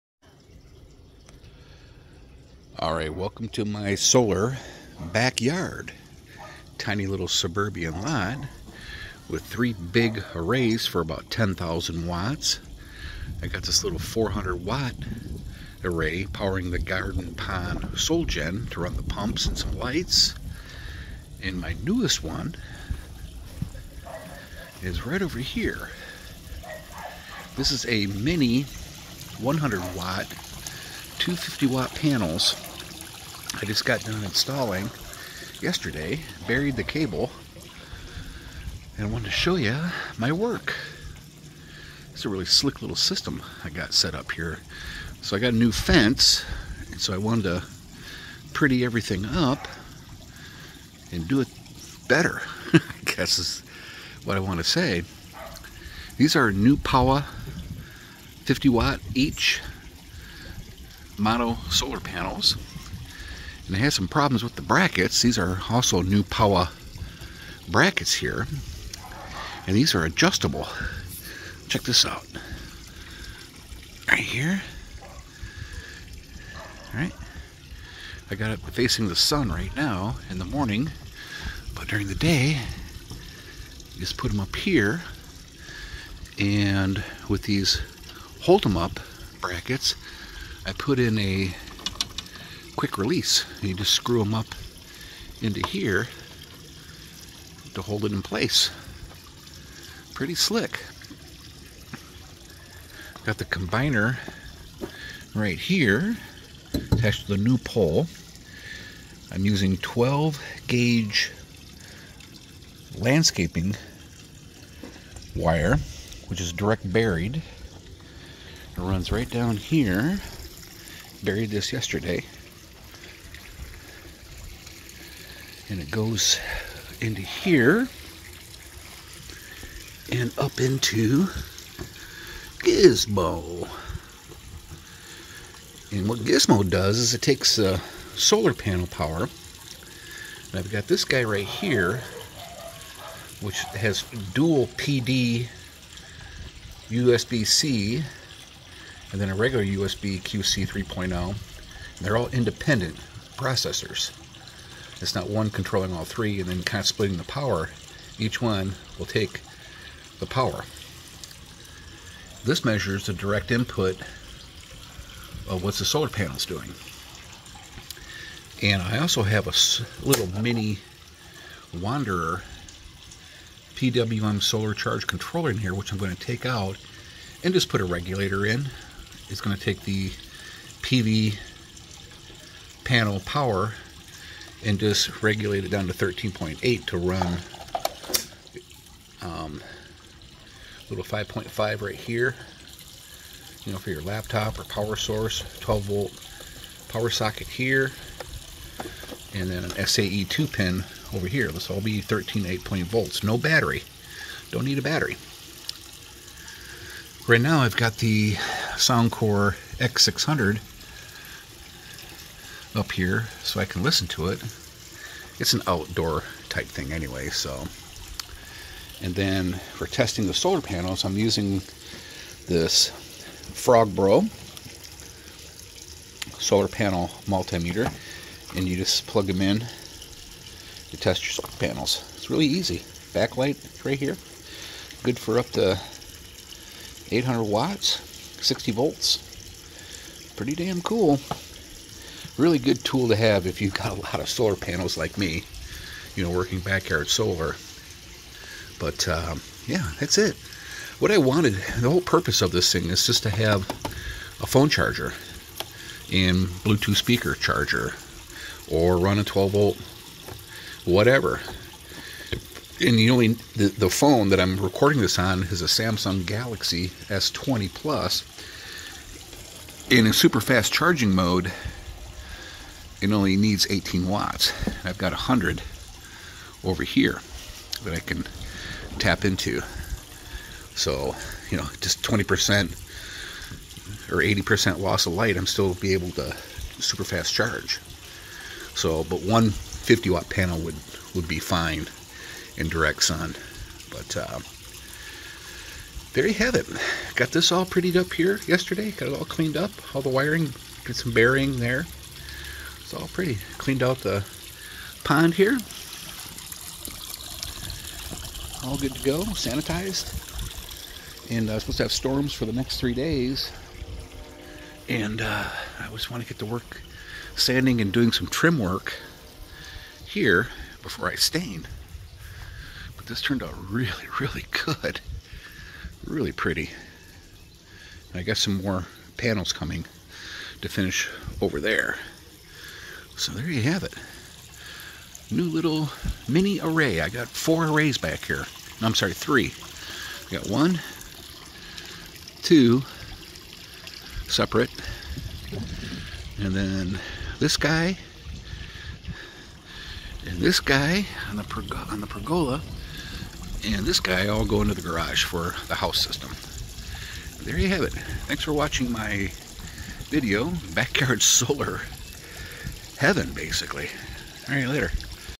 New Newpowa 100w mini solar panel array -- mounted and working!
I have been wanting a "mini" backyard solar panel array for years. I have always used my 100w Renogy solar panel suitcase if I wanted to play around, charge a phone, etc. However, lots of cords, wires, and hassle. So, I installed this Newpowa 12v mono 50w (x2) fence mount array and buried the 12 AWG landscaping wire into my "GIZMO" power converter I made.
NEWPOWA 50w 12v mono solar panel
NEWPOWA Adjustable Solar Panel Tilt Mount Brackets Kit

Chris DIYer Amazon Store: